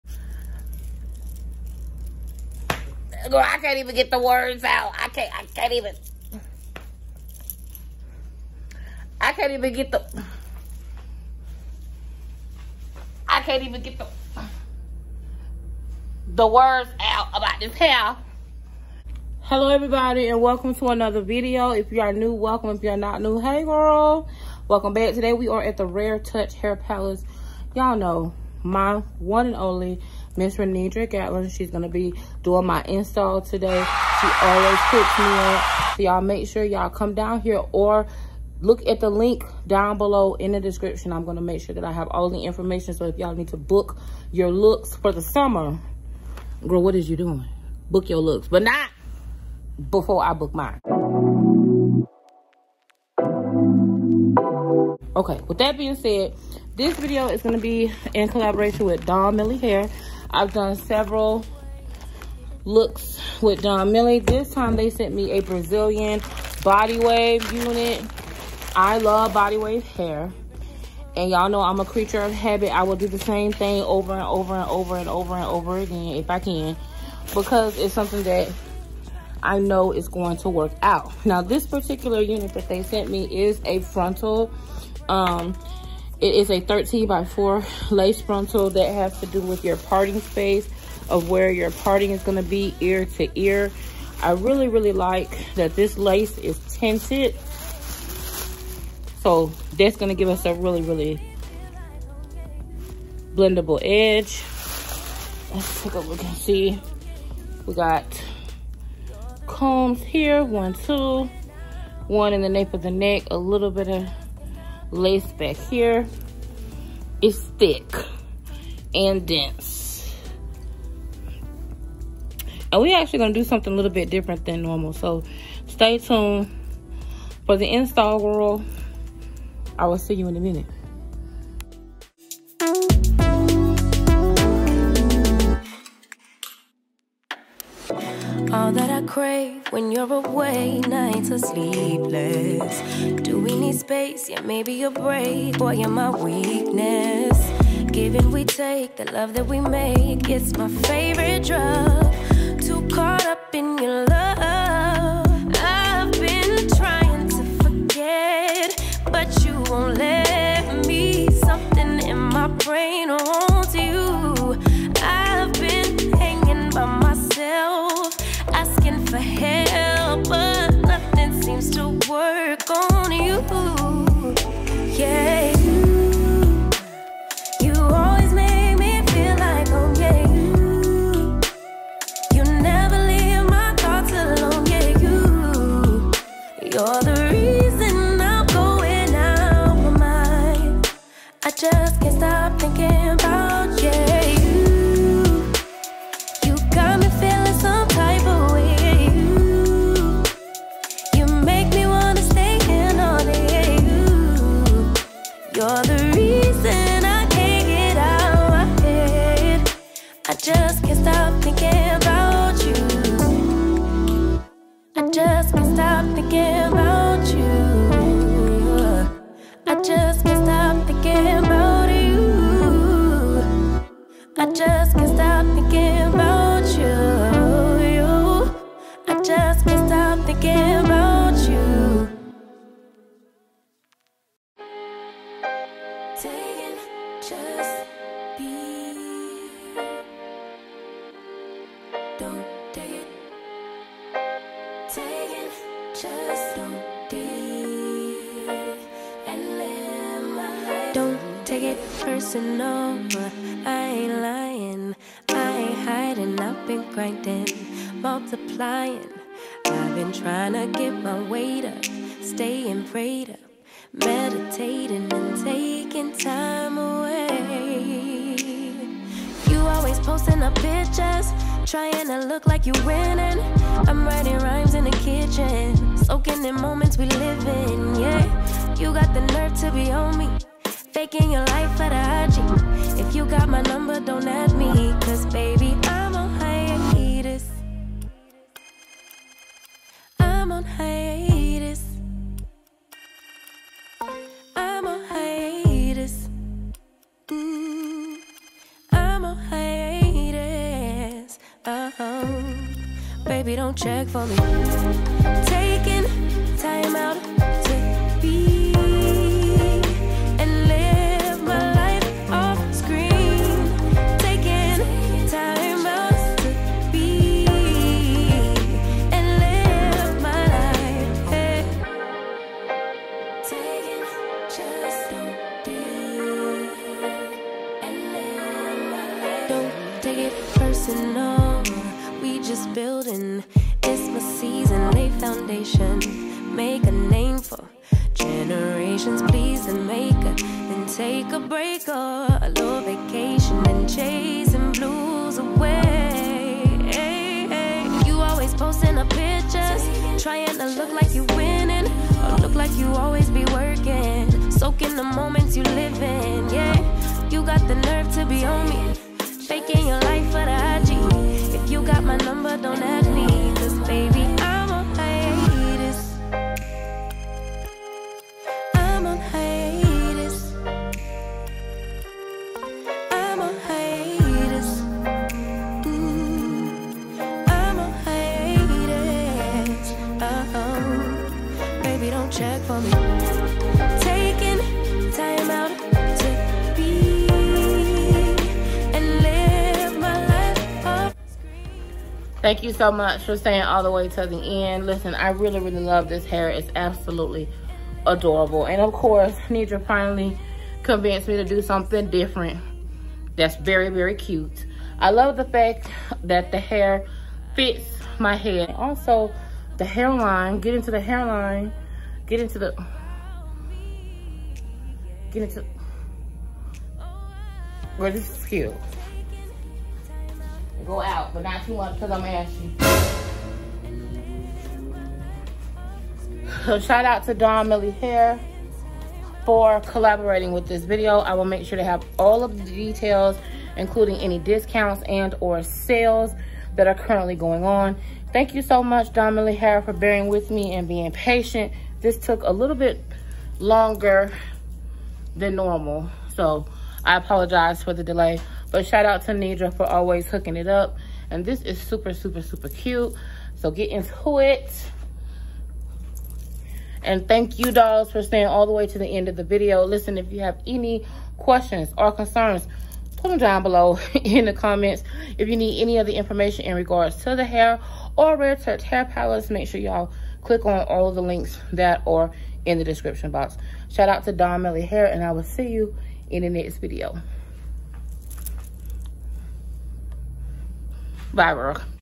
Its The Top Bun For me | Gots 2 Be Glued or Ebin Lace Glue | Donmily Hair | Joy Amor
Hey Dolls we have inches and inches of 13x4 Lace Frontal Body Wave 24 inches !!!No more Gots 2 be Glued !!! Ebin Lace Glue is the Goat ... Its the Top Bun for me !! Let me know if you are feeling this look.

Hair Info: Donmily 13×4 Lace Frontal Body Wave Hair 24”

Donmily Graduation Season Sale is Rocking On the Way!!!

if you love Travel definitely check out our plus size travel